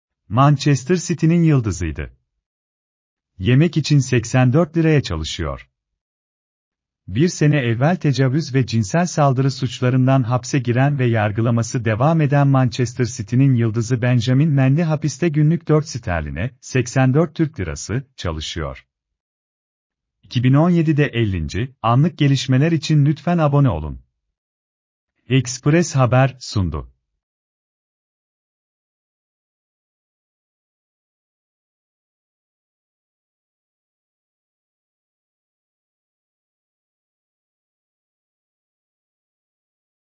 Manchester City’nin yıldızıydı! Yemek için 84 liraya çalışıyor
1 sene evvel tecavüz ve cinsel saldırı suçlarından hapse giren ve yargılaması devam eden Manchester City’nin yıldızı Benjamin Mendy hapiste günlük 4 Sterlin’e (84 TL) çalışıyor. 2017’de 50.